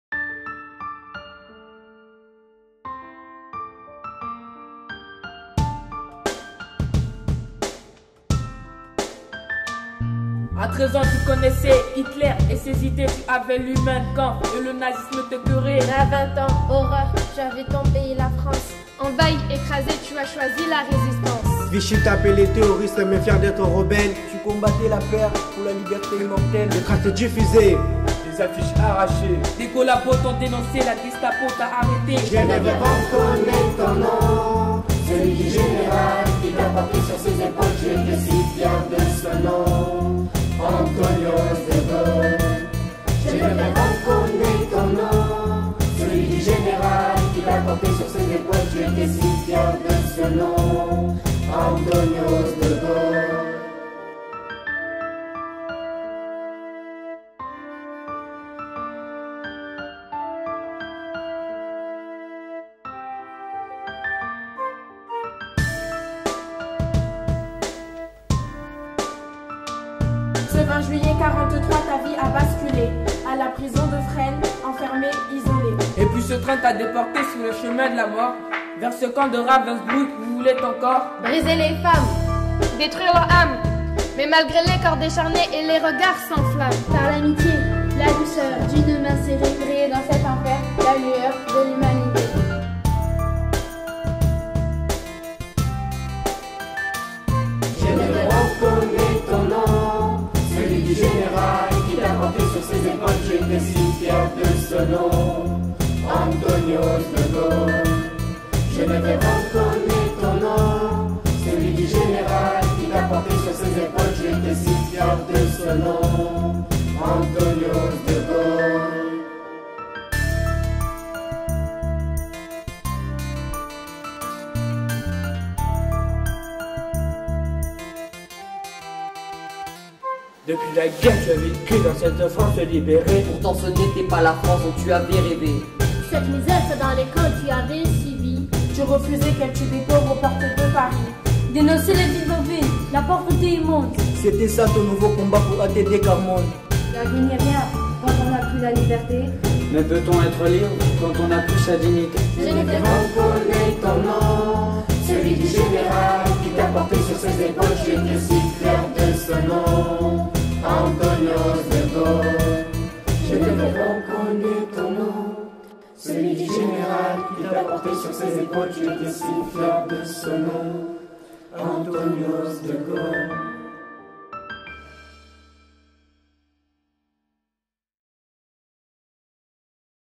Concours Panthéon 2015 // Collège de Cenon, académie de Bordeaux
Le Collège Jean Zay de Cenon a réalisé une vidéo dans le cadre du concours Panthéon 2015 organisé par l'Education nationale autour de Geneviève de Gaulle Anthonioz.